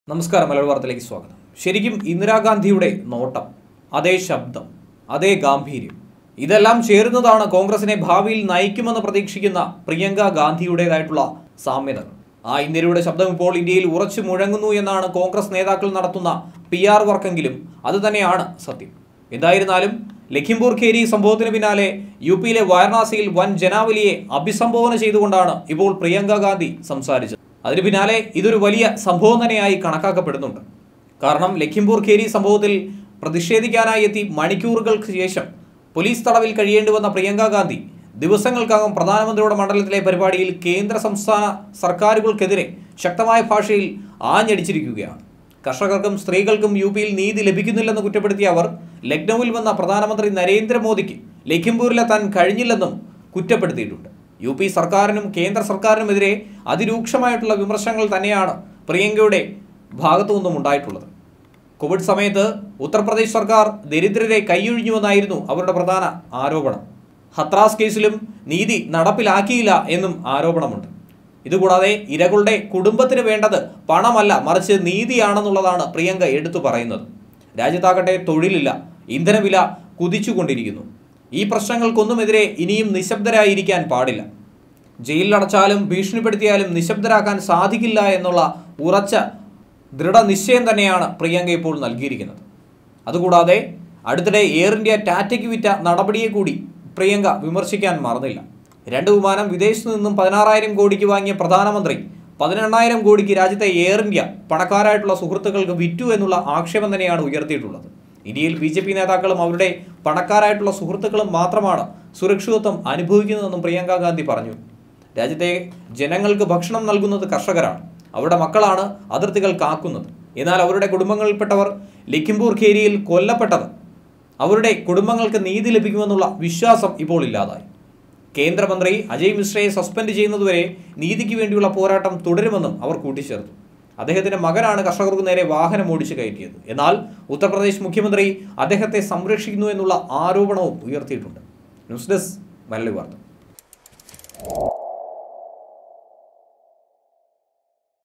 Priyanka Gandhi മോദിയുടെ മണ്ഡലത്തിൽ തകർപ്പൻ പ്രസംഗം
"ഇന്ദിരയുടെ ശബ്ദം!
മോദിക്കും യോ​ഗിക്കും
എതിരെ ആഞ്ഞടിച്ച്
പ്രിയങ്കാ​ഗാന്ധി
കോൺ​ഗ്രസിന് തിരിച്ച് വരവ്
Indira# Priyanka# Modi# India